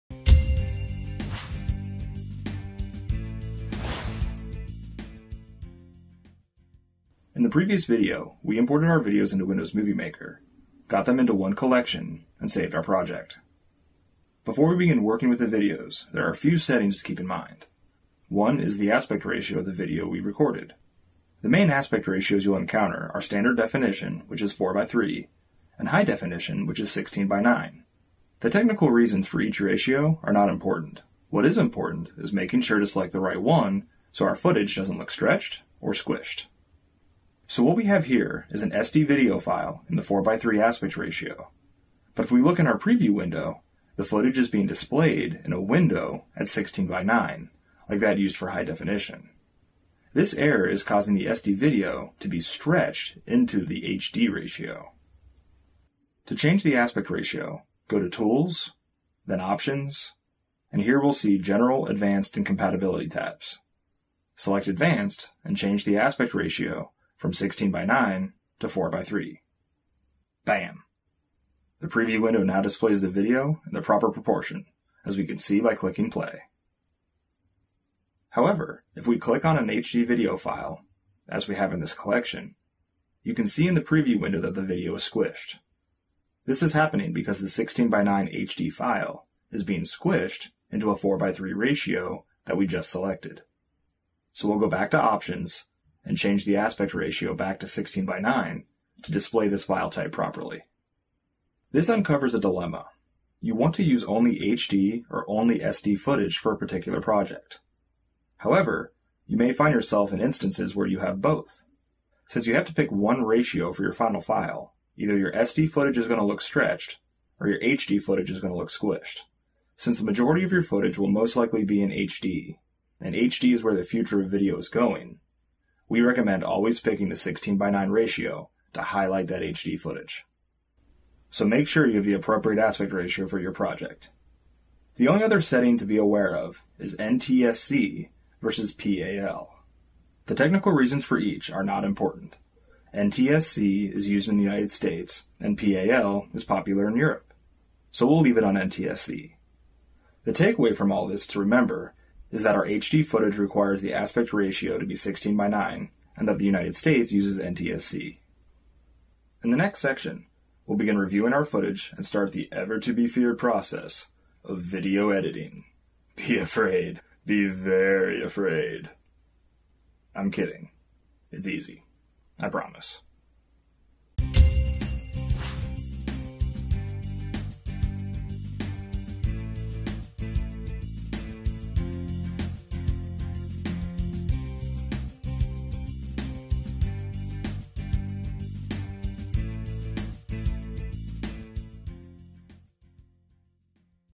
Video Settings in Windows Movie Maker
The video settings in Windows Movie Maker can ruin a project you working on if you unaware of their implication.  So if your footage is stretched or squished, then this tutorial is for you.  We cover how to change from standard to high definition formats as well as make sure that NTSC is selected rather than PAL.